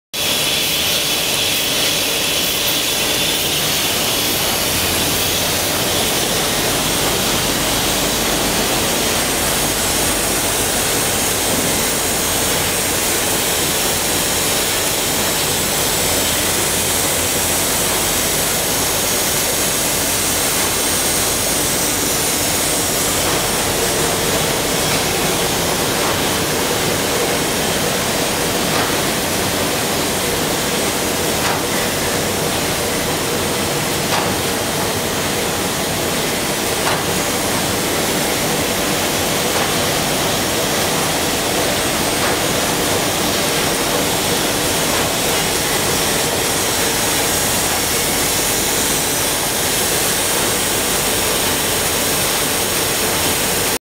Tymi Powder Cleaning Machine PC-126BH
PC-126BH is equipped with brush suction and heated cleander two units of powder cleaning devices for more effective performance to remove priting powder.